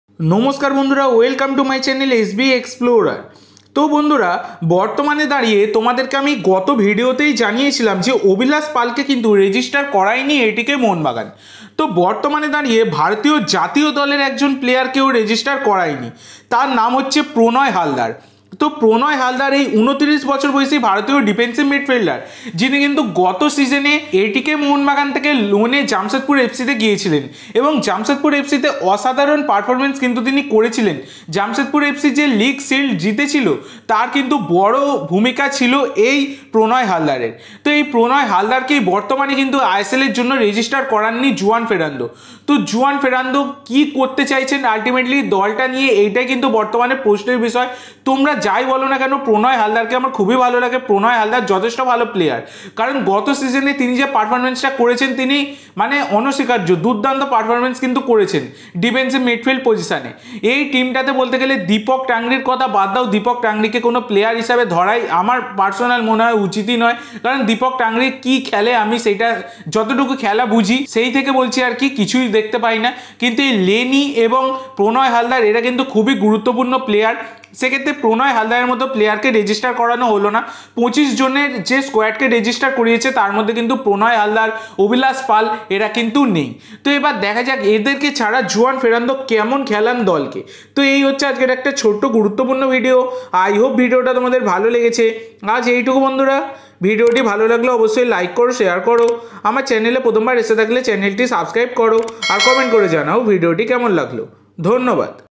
🔴জাতীয় দলের প্লেয়ারকে স্কোয়াড থেকে বাদ দিলেন জুয়ান | Atkmb News | Atk Mohunbagan
Comment ⌨️ করুন।

ধন্যবাদ🙏🙏🙏🙏🙏